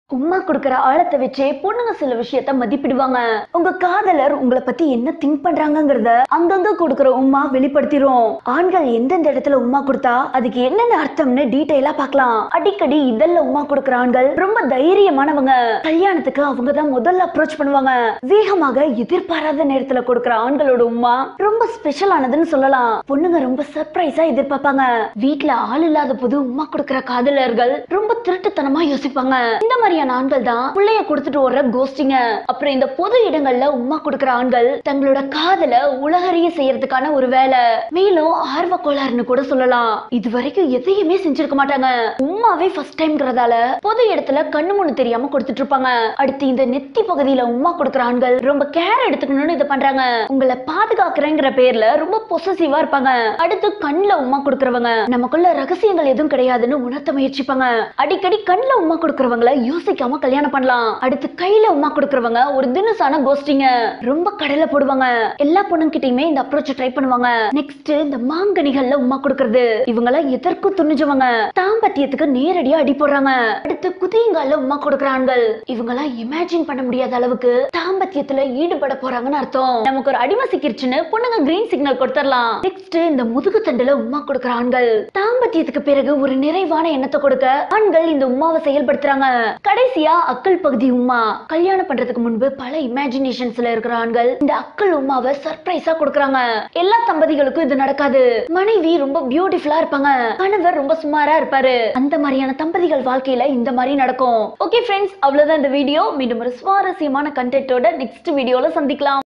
உம்மா கொடுங்க - Strong foundation for marriage life
Have you ever wondered why some marriages seem to flourish while others
seem to continually flounder?

Do you have a desire to make your own marriage stronger and more connected?

There is a wealth of research identifying the traits that make a marriage
strong, resilient, and satisfying. I want to share three very important
qualities needed to build a strong foundation for marriage.

A strong foundation for marriage

Research tells us that strong marriages have high levels of friendship and
emotional connection at their foundation